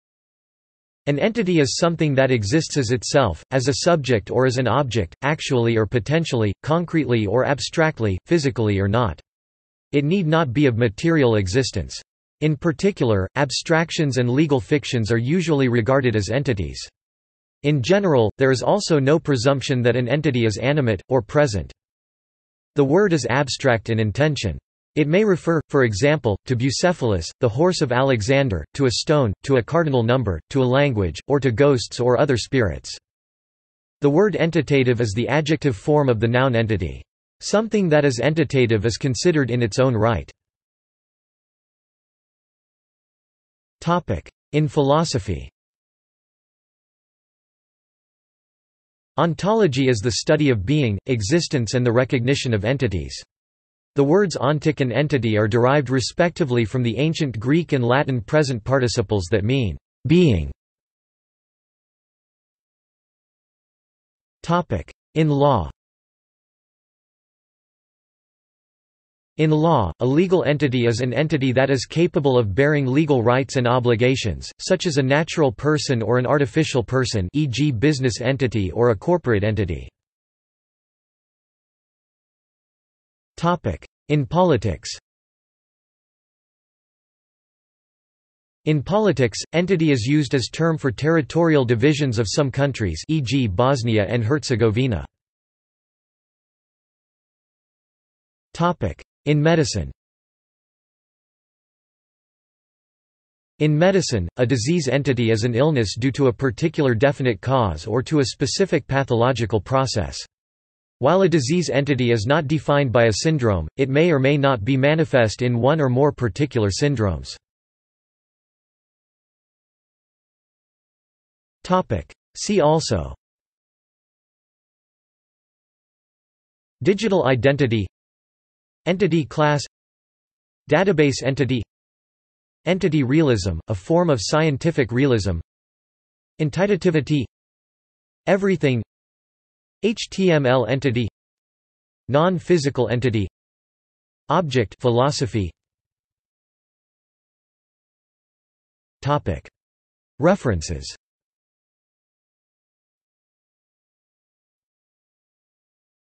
Entity | Wikipedia audio article
This is an audio version of the Wikipedia Article:
Entity

- Socrates

SUMMARY
An entity is something that exists as itself, as a subject or as an object, actually or potentially, concretely or abstractly, physically or not.  It need not be of material existence. In particular, abstractions and legal fictions are usually regarded as entities. In general, there is also no presumption that an entity is animate, or present.
The word is abstract in intention. It may refer, for example, to Bucephalus, the horse of Alexander; to a stone; to a cardinal number; to a language; or to ghosts or other spirits.
The word entitative is the adjective form of the noun entity. Something that is entitative is considered in its own right.